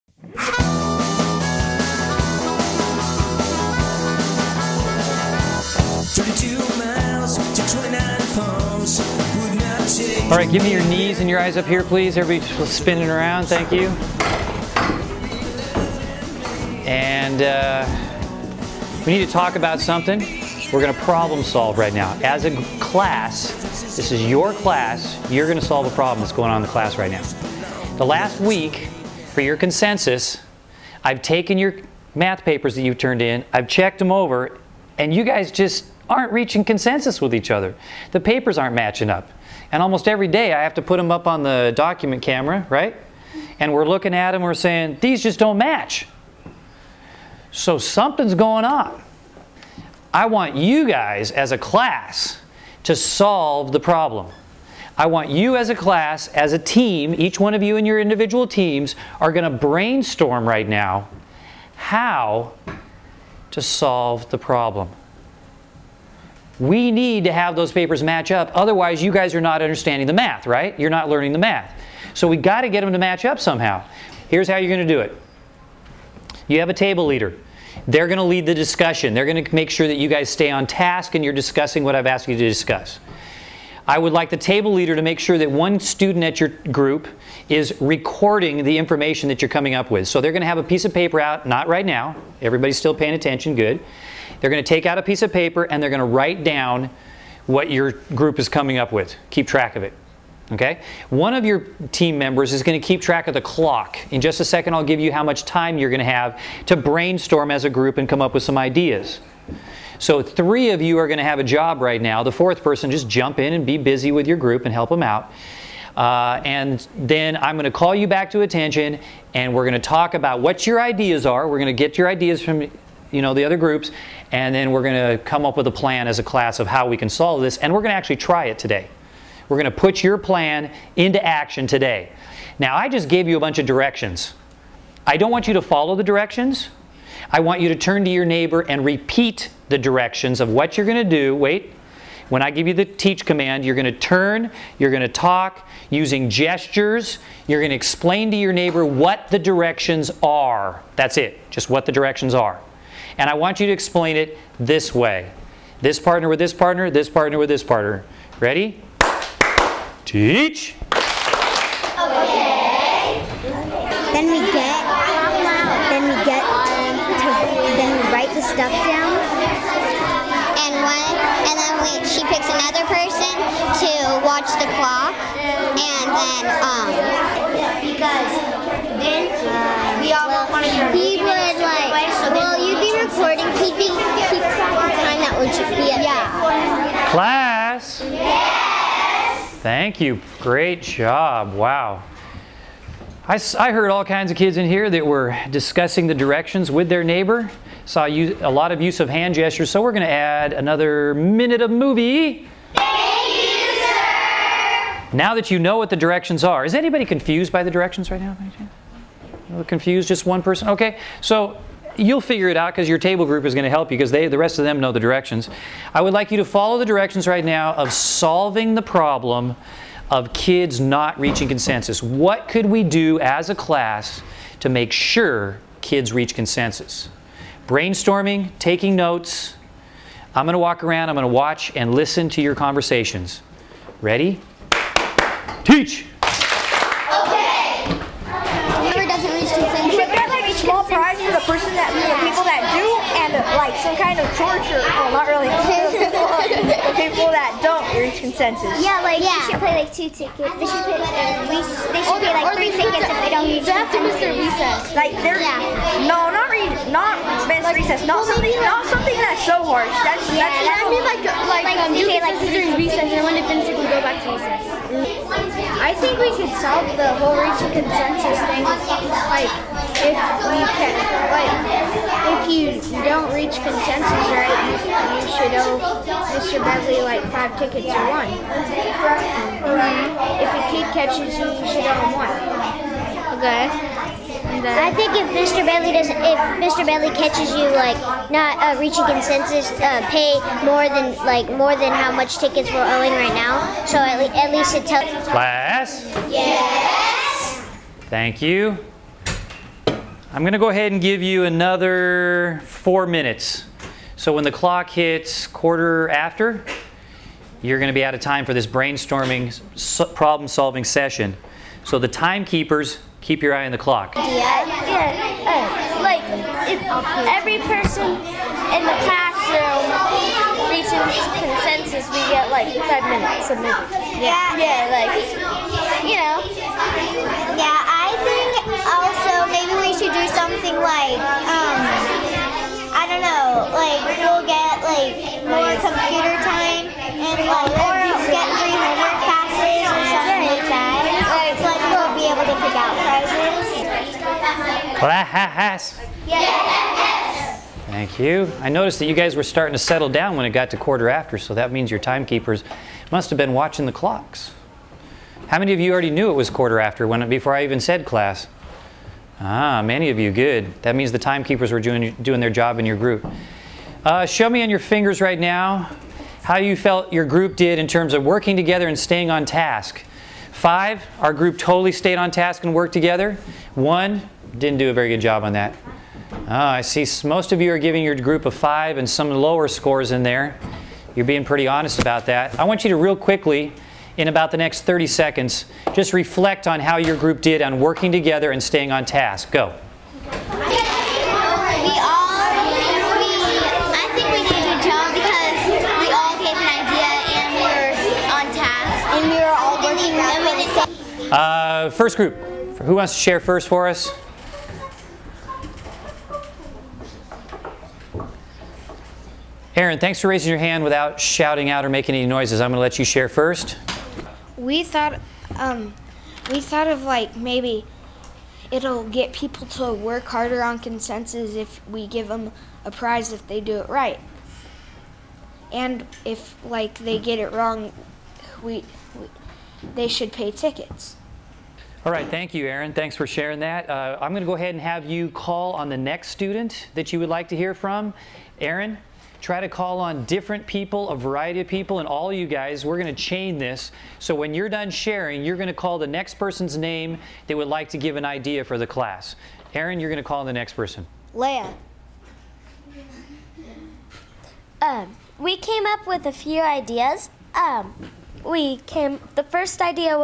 Model Lesson: Student Engagement with Tim Bedley (pt. 1 of 3)
This is a lesson taught by Tim Bedley, 2013 Riverside County Teacher of the Year, as he leads his students to come up with their own solution to an issue that has arisen in the first week of school.
Captions highlight Tim's infusion of the California Standards for the Teaching Profession (CSTP's) throughout the video, making this the perfect tool for training teachers.